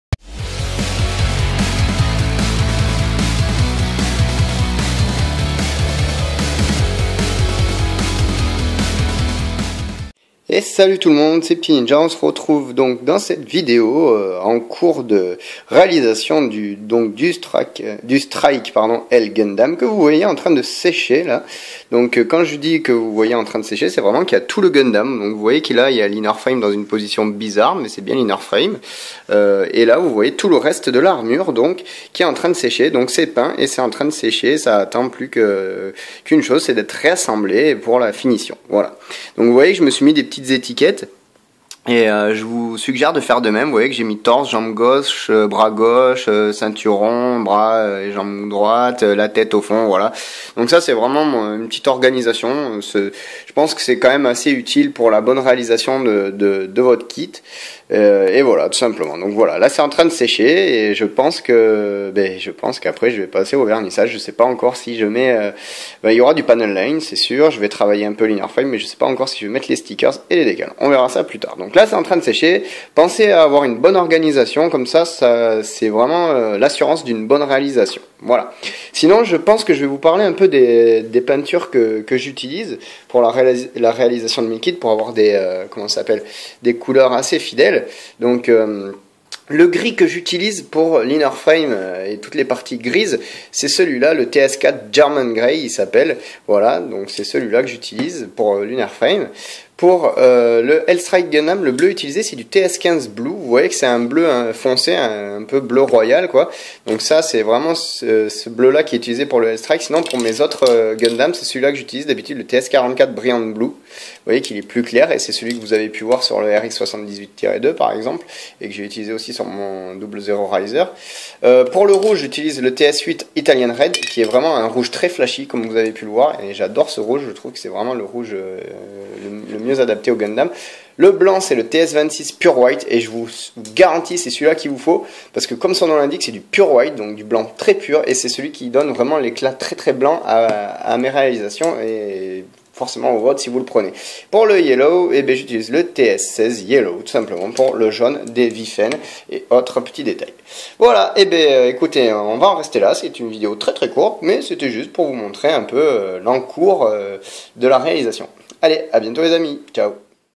Couleurs de base [Tuto]
Salut à tous, dans cette vidéo je vais vous présenter les différentes références de couleurs en bombe Tamiya que j'utilise pour mes réalisations. Enjoy !